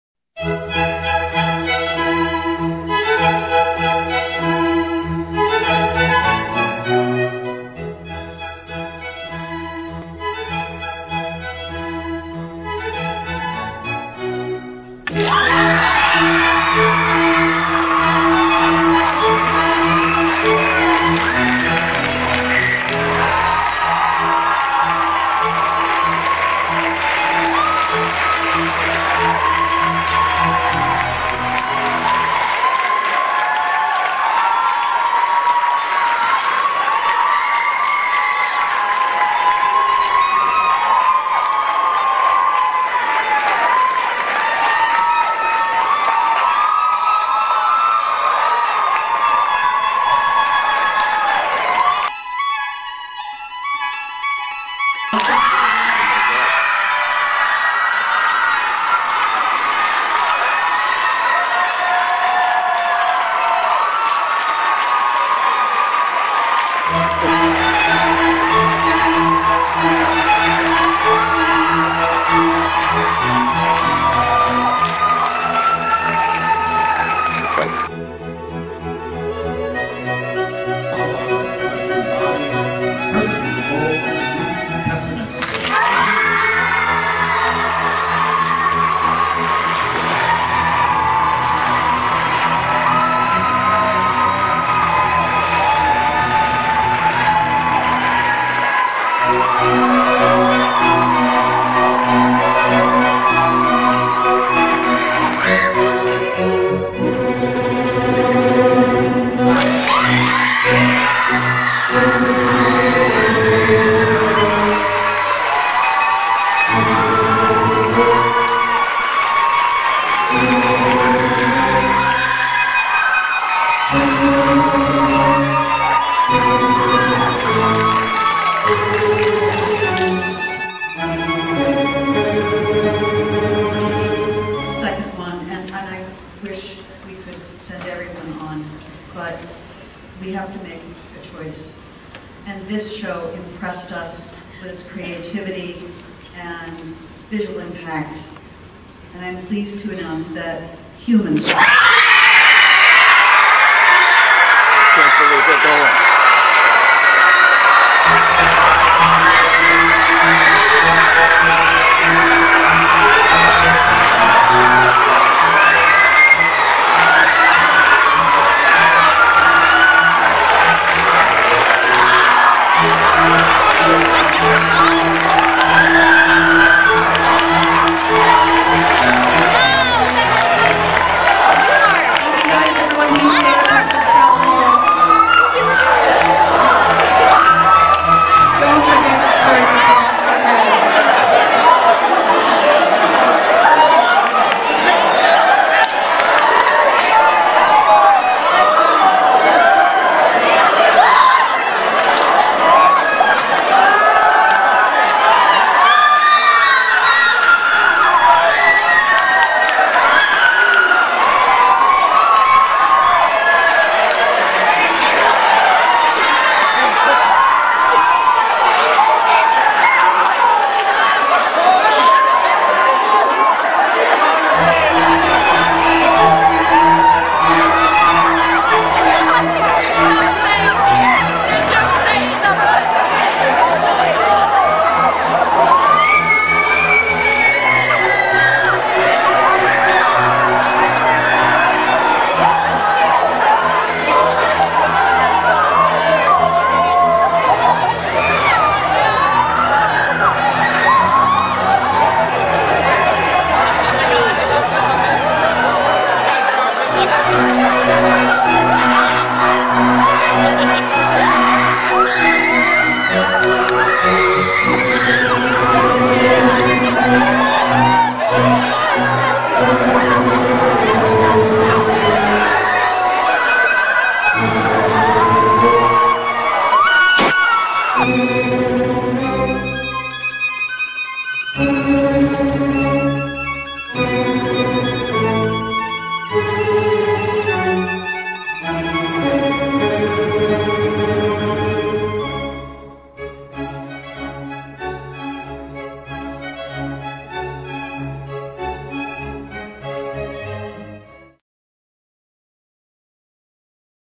Holliston High School "Humans" 2012 MA Drama Festival - Semi Finals Awards Video
Holliston HS, Massachusetts Drama Festival combined clips of award winnners at Semi Finals.

plus photos from backstage/in makeup at semis are also uploaded
- set to free share but if you run into problems/want copies let Alessandro know

James Arena-DeRosa